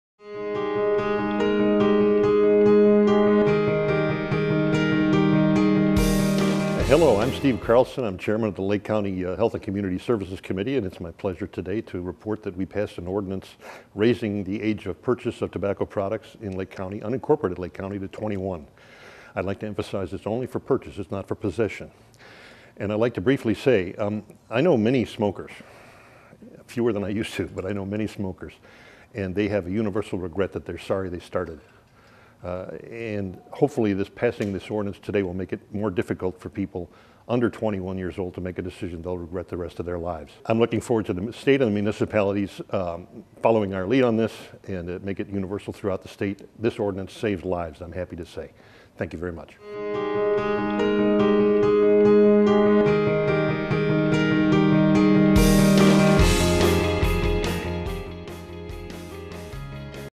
County Board Action: Lake County Raises Tobacco Purchase Age to 21
Lake County recently became the first County in Illinois to ban the sale of cigarettes, e-cigarettes and tobacco products to anyone under the age of 21 in the incorporated areas of the County.